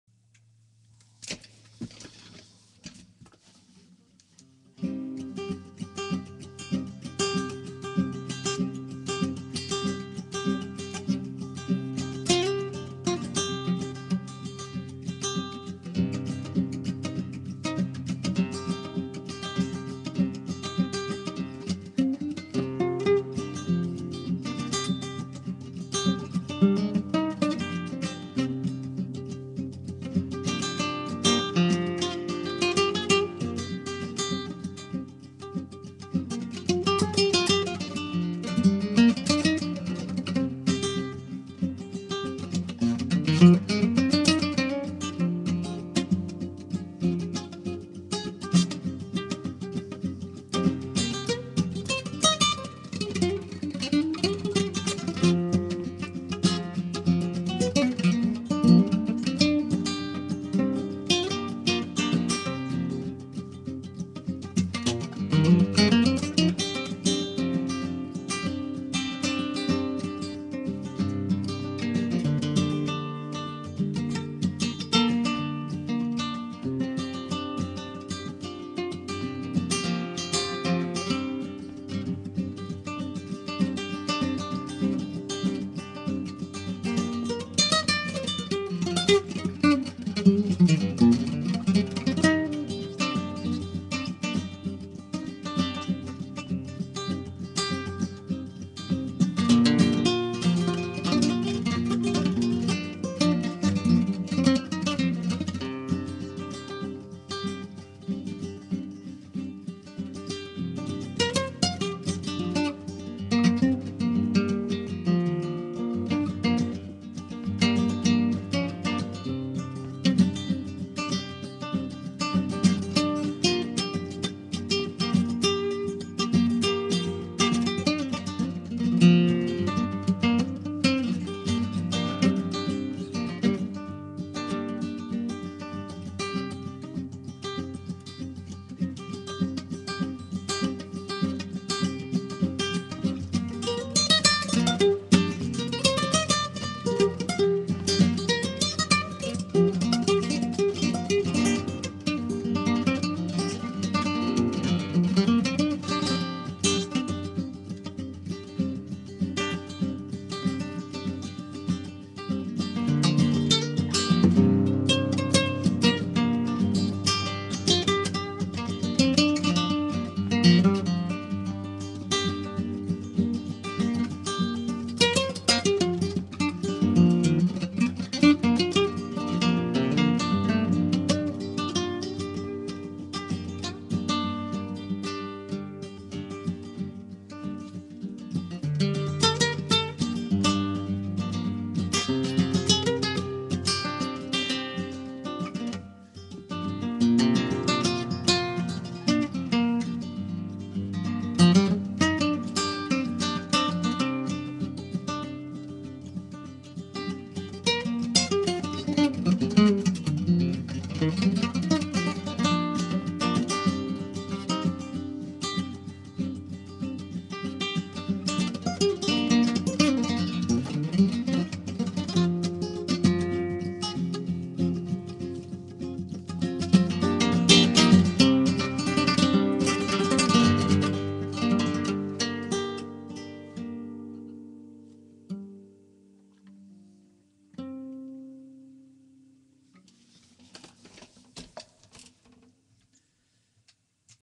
sparks
Guitarbastardo records a short guitar improvisation live via YouTube webcam every morning, without rehearsing anything in advance or cosmetic editing afterwards.